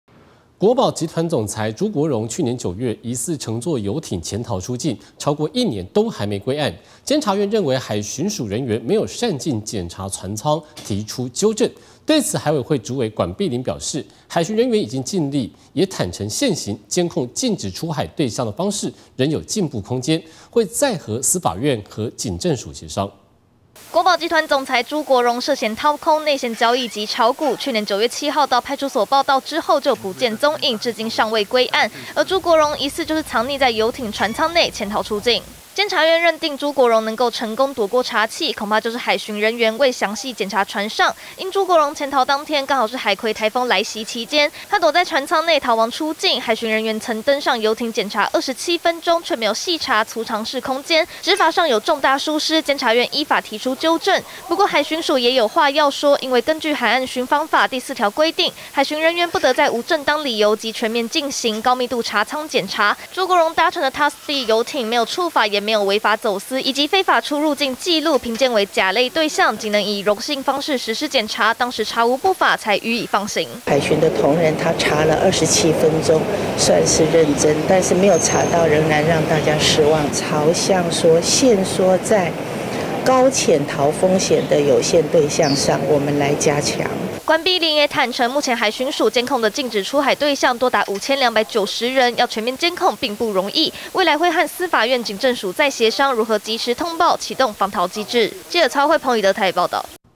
登船檢查朱國榮仍順利潛逃 監院糾正海巡署
國寶集團總裁朱國榮，去年9月疑似乘坐遊艇潛逃出境，超過一年都還沒歸案，監察院認為海巡署人員沒有善盡檢查船艙，提出糾正。對此海委會主委管碧玲表示，海巡人員已經盡力，也坦承現行監控禁止出海對象的方式，仍有進步空間，會再和司法院和警政署協商。